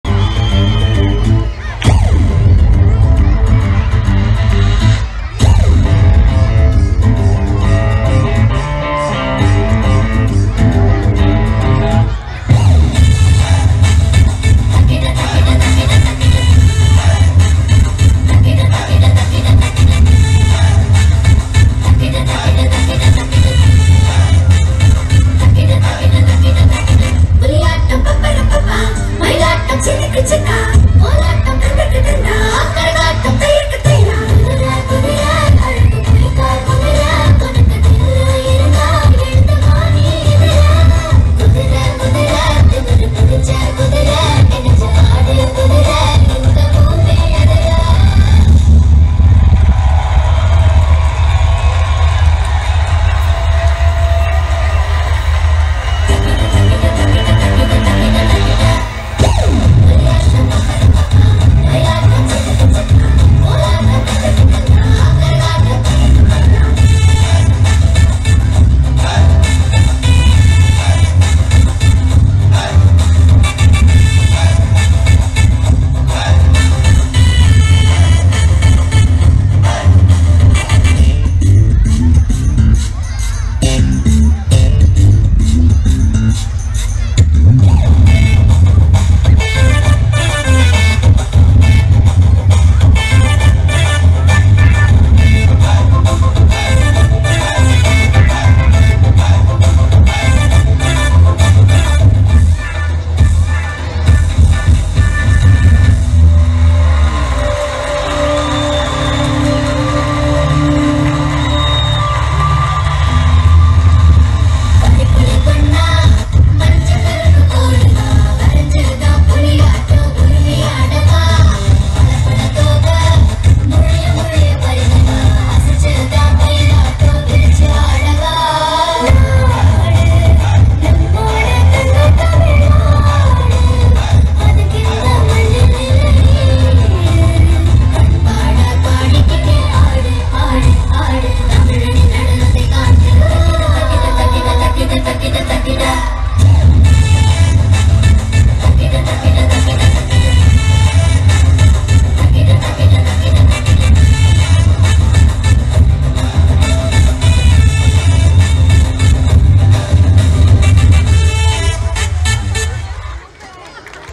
Lakshmi dance performance by Gr1
Hayagrivar Vidyaashram.Sr.Sec.School, Arakkonam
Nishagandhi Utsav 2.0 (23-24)